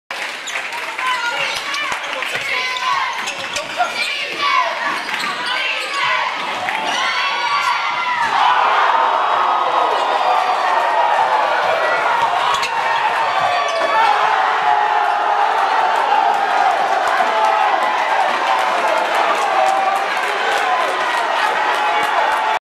APBL Final Four Metro AllStars vs Rockville Victors Deontay Twyman & Armond Hill Dunks-FinestMag.com
Unbelievable sequence of dunks during the APBL Final Four match-up between the Metro All Stars and Rockville Victors. Metro's 5'11 point guard Deontay Twyman flies in for a vicious dunk and Rockville's high flying 6'6 guard/forward Armond Hill immediately returns the favor with a nice flush off the lob.